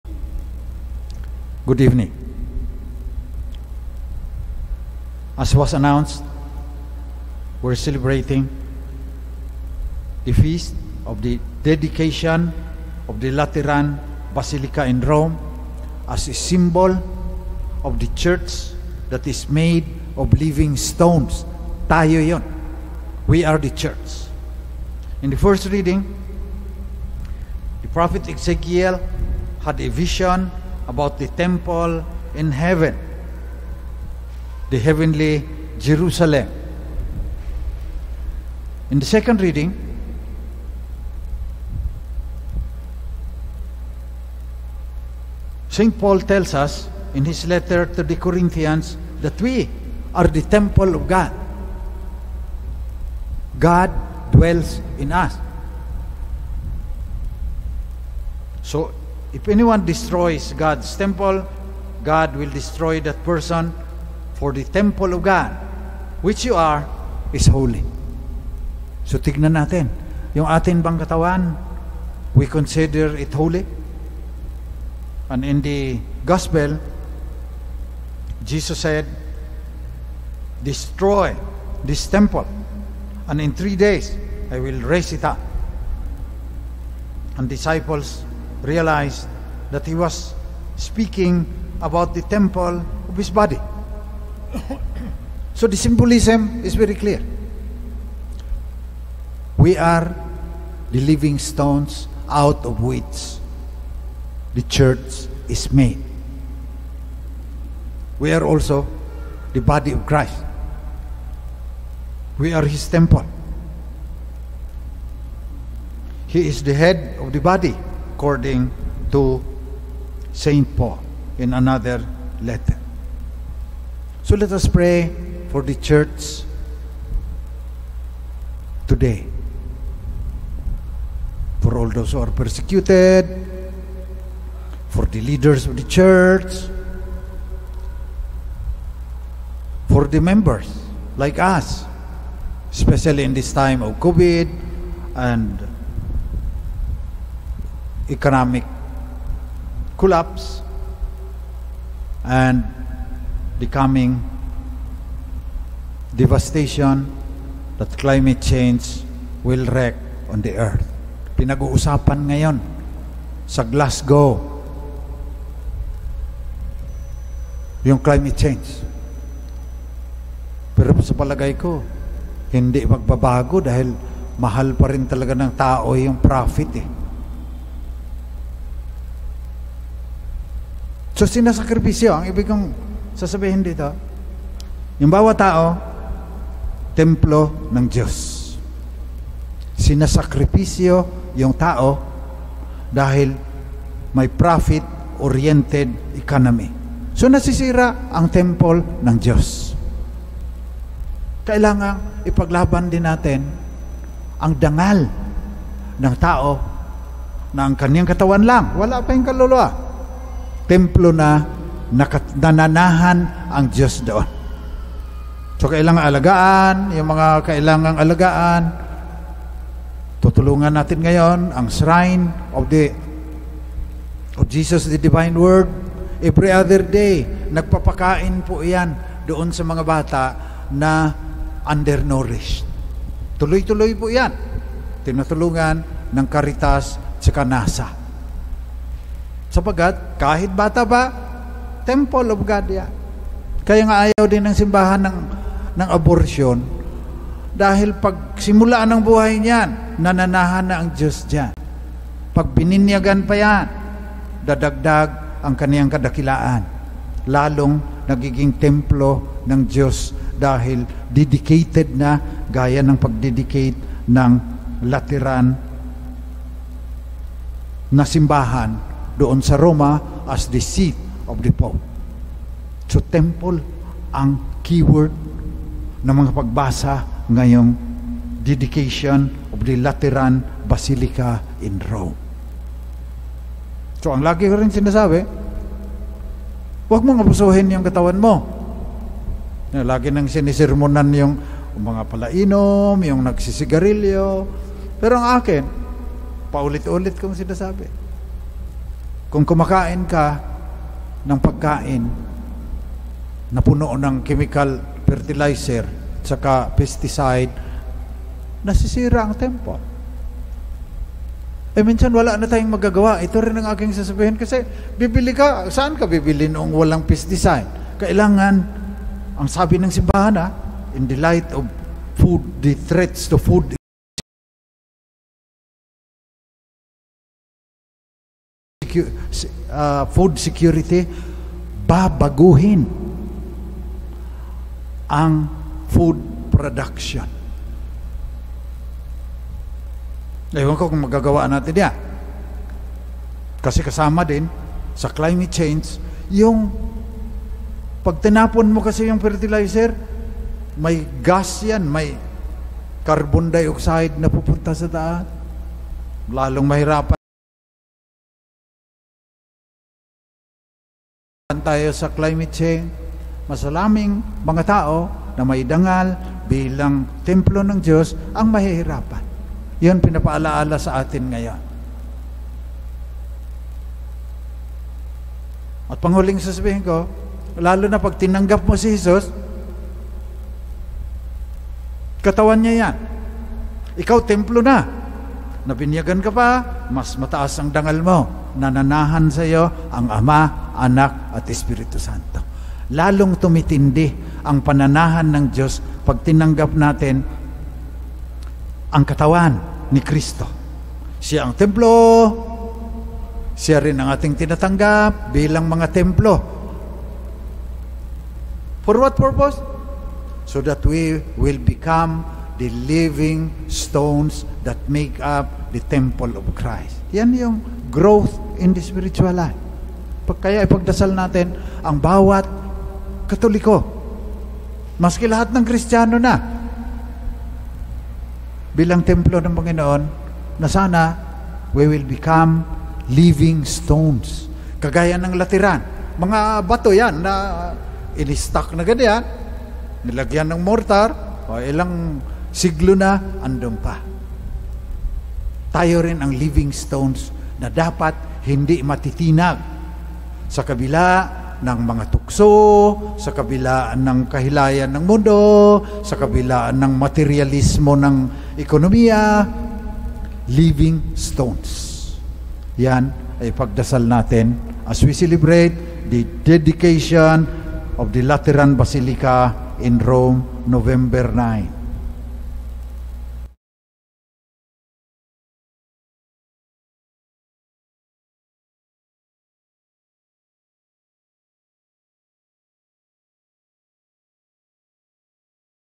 HOMILY OF REV. FR. BENIGNO BELTRAN, SVD
Holy Mass - 6:00pm, Tuesday,  November 9, 2021 at the Diocesan Shrine of Jesus the Divine Word, Christ the King Mission Seminary, Quezon City, Philippines.